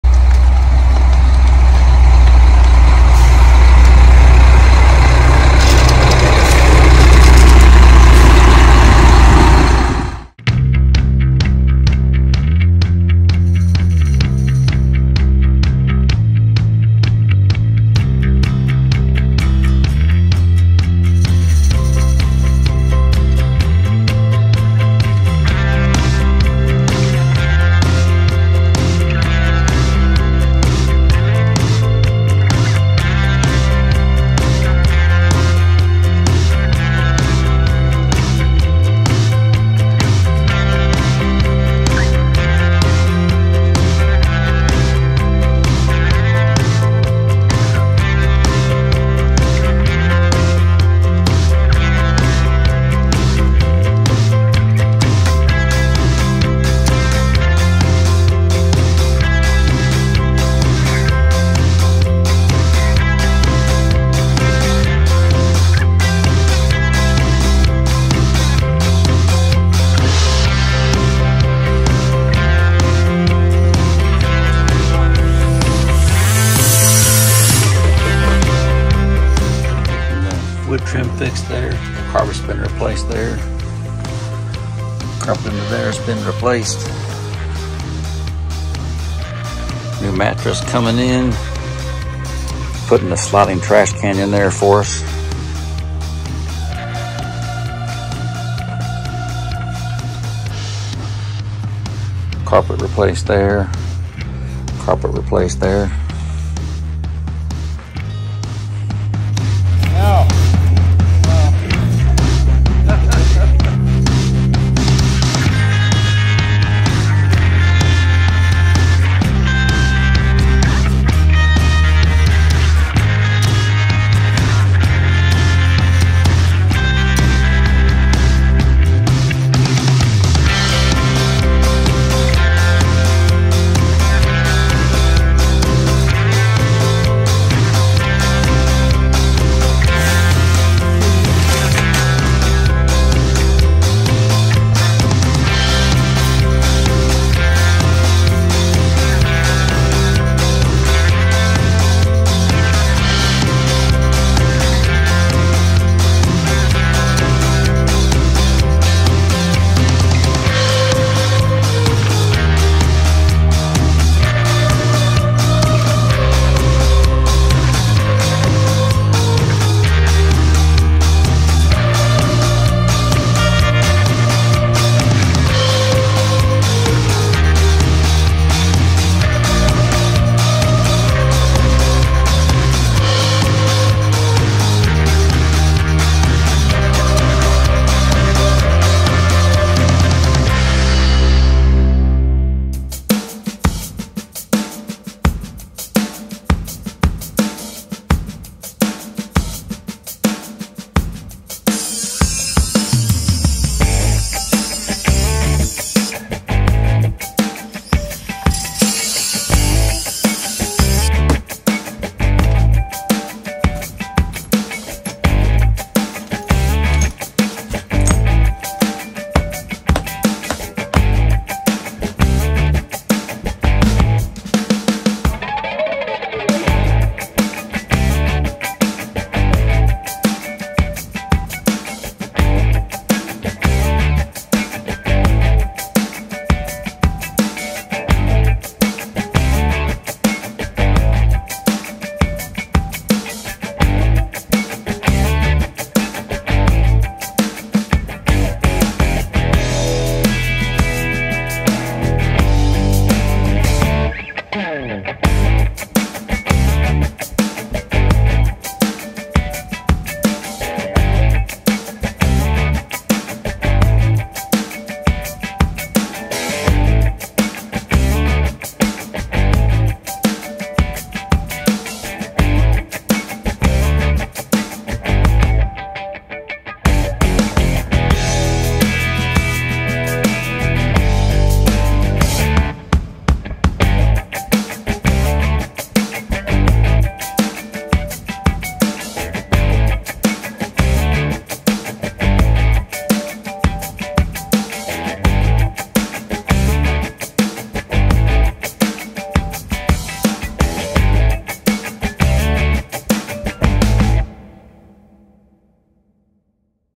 Paul's RV - "Coach Refresh"
We planned to do a "refresh" on our Travel Supreme several years ago.  We had a long list of items worked on, replaced or repaired.  This video is a short version of what was done.